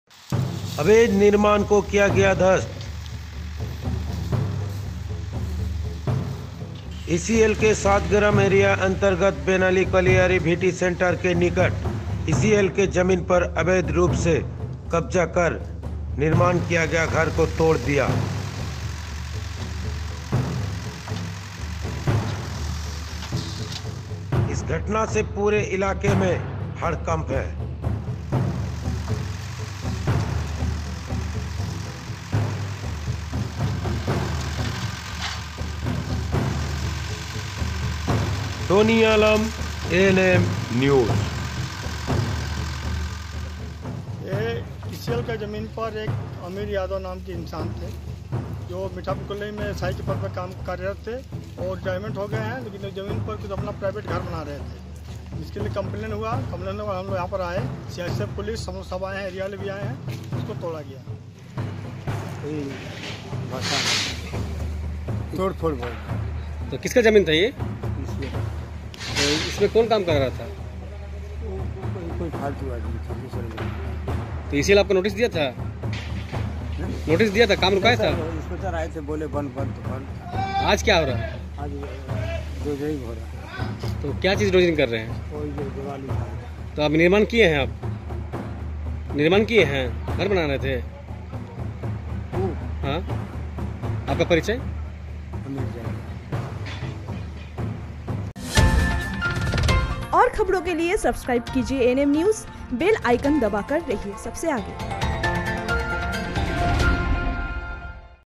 ECL : अवैध निर्माण को किया ध्वस्त
टोनी आलम, एएनएम न्यूज़ : ईसीएल के सातग्राम एरिया अंतर्गत बेनाली कोलियरी भीटी सेंटर के निकट ईसीएल के जमीन पर अबैध रूप से कब्जा कर निर्माण किया गया घर को तोड़ दिया। इस घटना से पूरे इलाके में हड़कंप मच गया। गुरुवार को ईसीएल के सातग्राम एरिया के सुरक्षा अधिकारी चंद्रकेश हरिजन के नेतृत्व में श्रीपुर फाड़ी पुलिस के एसआई दयामय मुखर्जी और एबीपिट सीआईएसएफ जवानों के मौजूदगी में जेसीबी मशीन द्वारा निर्माण को तोड़ दिया गया। इस घटना के संबंध में ईसीएल के सातग्राम एरिया के सुरक्षा अधिकारी चंद्रकेश हरिजन ने बताया कि खबर मिली थी कि ईसीएल के सातग्राम एरिया के बेनाली कोलियरी भीटी सेंटर के पीछे मीठापुर के एक सेवानिवृत्त एक सुरक्षा गार्ड ने ईसीएल के जमीन को अवैध रूप से कब्जा कर निर्माण कार्य कर रहा है। सूचना पाकर इस कार्य को रोक दिया गया था गुरुवार को पुलिस एवं सीआईएसएफ के सहायता से निर्माण कार्य को तोड़ दिया गया है। इस तरह कोई भी ईसीएल की जमीन पर निर्माण किया जाएगा तो उसको भी तोड़ा जाएगा।

वही कोलियरी के वरिय कार्मिक प्रबंधक संजीव घोष ने बताया कि हम लोगों को सूचना मिली थी कि ईसीएल के जमीन पर अवैध रूप से कब्जा हो रहा है। इसलिए हम लोगों ने सुरक्षा पर अधिकारियों को उस स्थान पर भेजा गया और जानकारी प्राप्त की गई पता चला कि वह जमीन ईसीएल का है। इसलिए मीठापुर के एक सेवानिवृत्त एक सुरक्षा गार्ड को नोटिस किया गया था कि किस वजह से जमीन को कब्जा किया गया इस जमीन के दस्तावेज दिखाए। लेकिन वह कोई भी कागज दिखाने में असमर्थ था इसलिए ईसीएल द्वारा कार्रवाई करते हुए उनके निर्माण कार्य को ध्वस्त कर दिया गया है। अगर उस जमीन पर कोई भी निर्माण के कार्य किया जाएगा तो उनके विरुद्ध कानूनी कार्रवाई किया जाएगा।